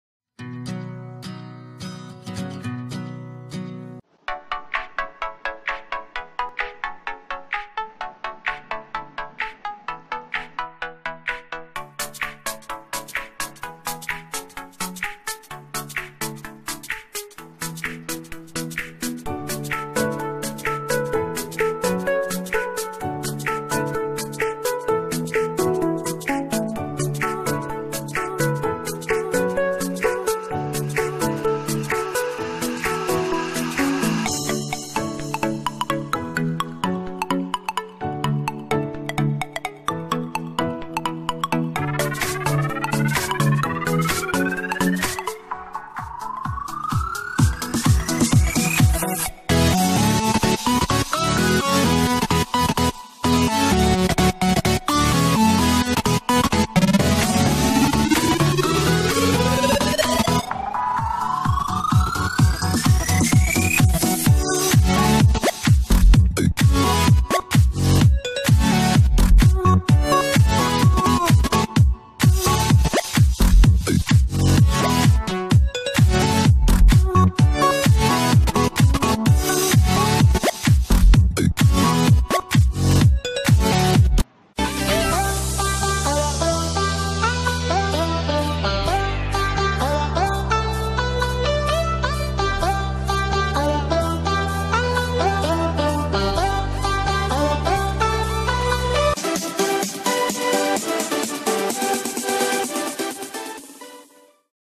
Electrical conductivity of water
Hello friends I'm Rishabh and you are watching RR Experiments attack and in this video I will show you conductivity of water and salt water. The water that we get from different sources, like tap, hand pumps, wells and ponds, generally contains several salts dissolved in it.This water is thus a reasonably good conductor of electricity But pure water is not good conductor of electricity.I hope guys you like this video.

STAY IN TOUCH AND KEEP WATCHING!!!